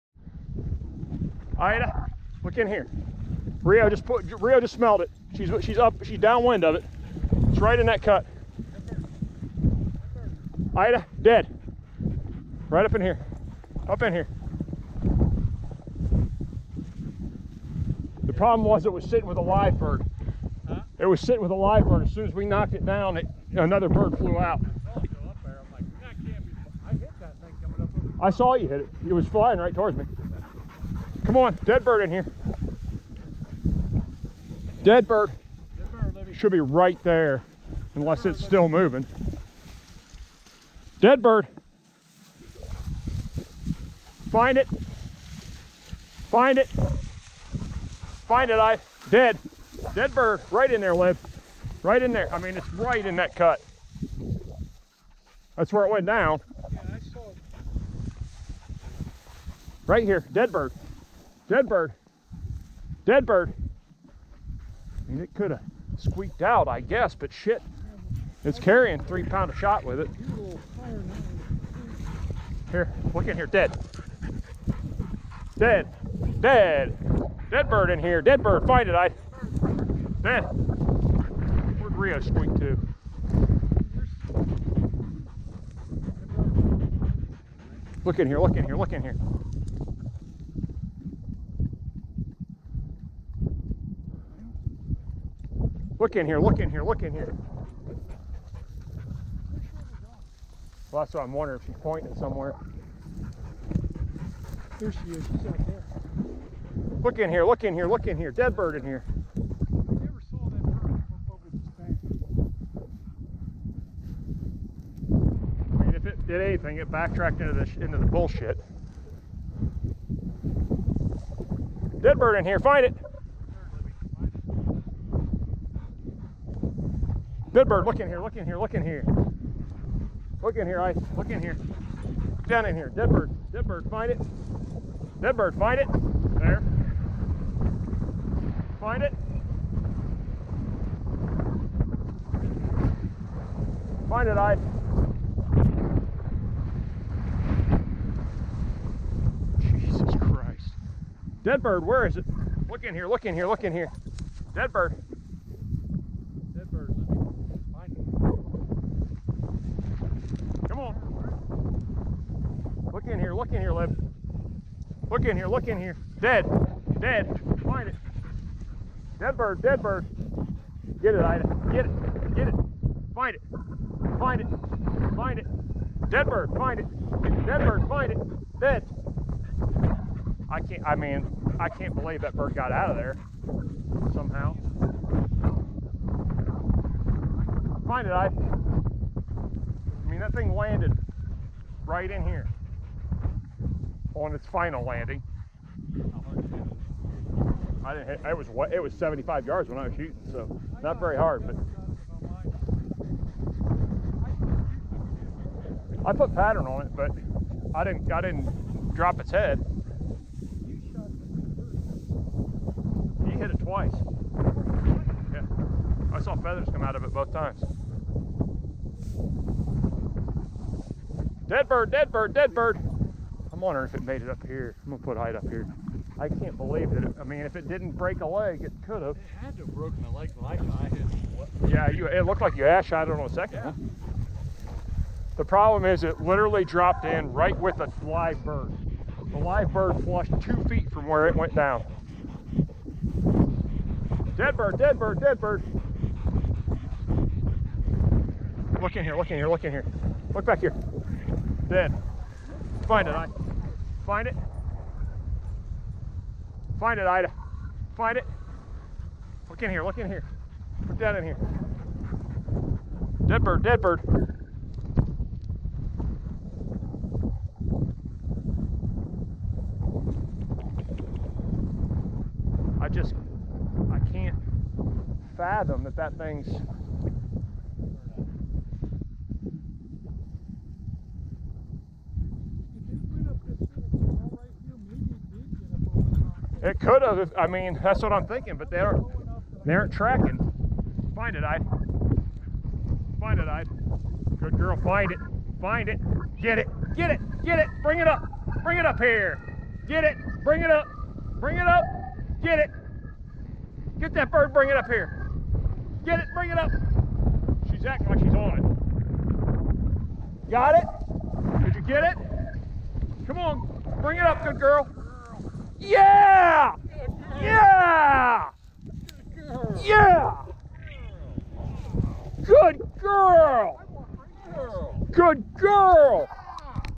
Want to know why hunting with bird dogs is the best? — Ultimate Upland
Windy, warm day with minimal shooting opportunities. Pointing, trapping and flushing educated birds is great. BUT if I'm honest, I'm never more excited than seeing a lost bird retrieved. And I let the dogs know.

This stuff isn't easy in these conditions. Watch and you can see how many times dogs and hunters walk past, around, over this bird.

You may be saying, how is this possible? The short answer is that this bird landed right next to where another live bird flushed (and probably many more live birds had run). It's tough for the dogs to try and weed through the scent and figure it out.